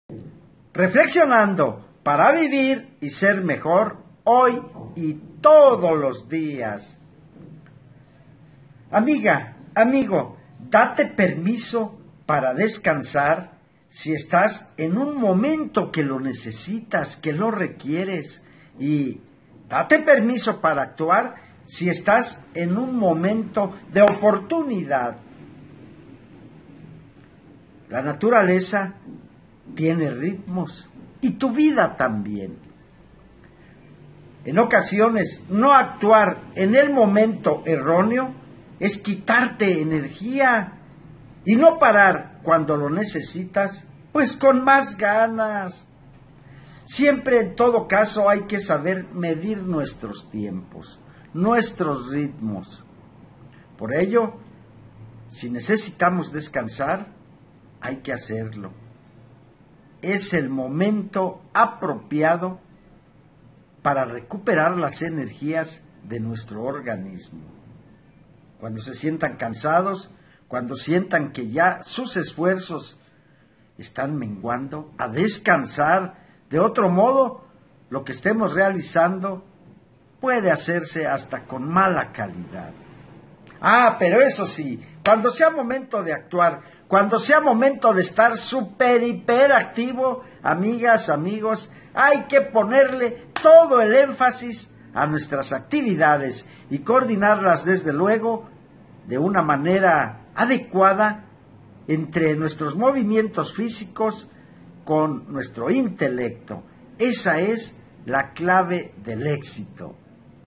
Coordinar el intelecto con los movimientos físicos, clave del éxito
Reflexiones con Francisco Camarillo López